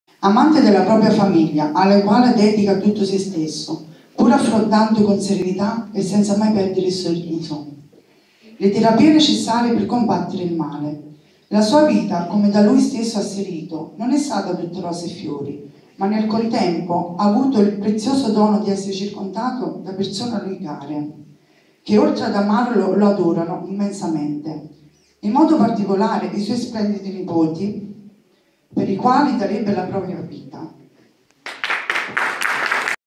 'Siamo Tutti Uno' - prima testimonianza
Presentazione del calendario dell'associazione 'Siamo tutti uno' nella biblioteca comunale di Cellole. Coloro che hanno vinto la battaglia contro il brutto male hanno portato la loro testimonianza di speranza